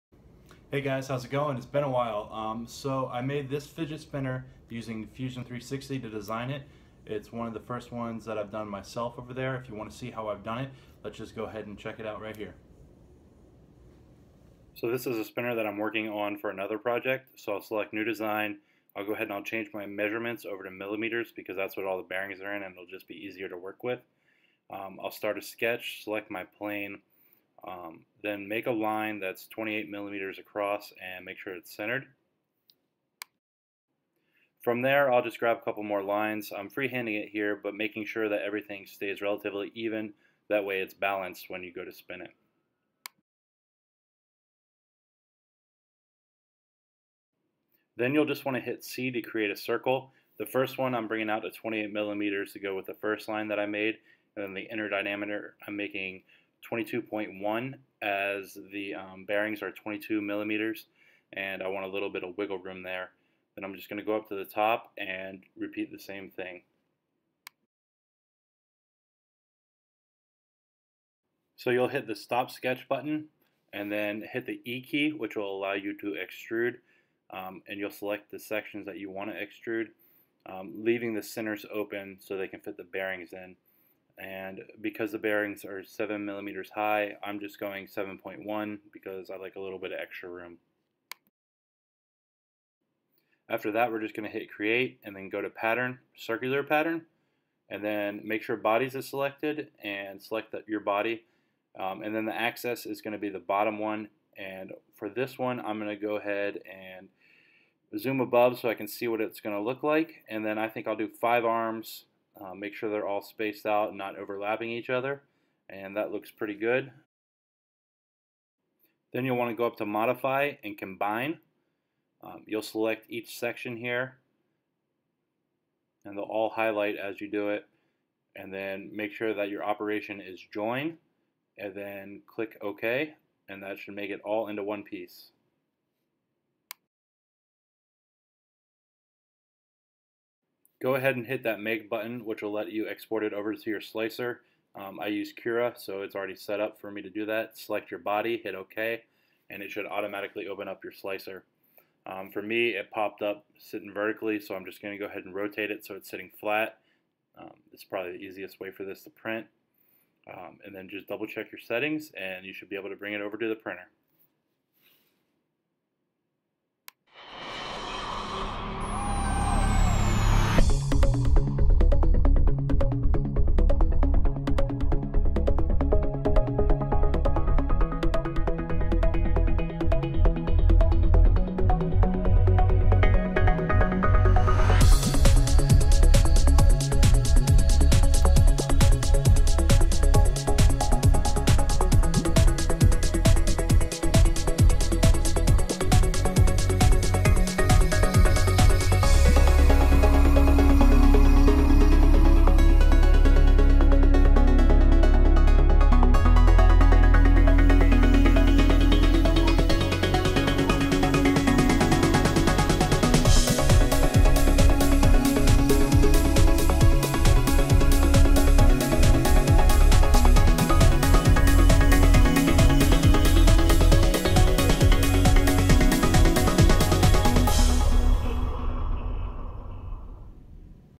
How To: Design a 3D Printed a Fidget Spinner Using FUSION 360
Fusion 360 is an easy tool to learn and worked great for designing this 3D printed Fidget Spinner.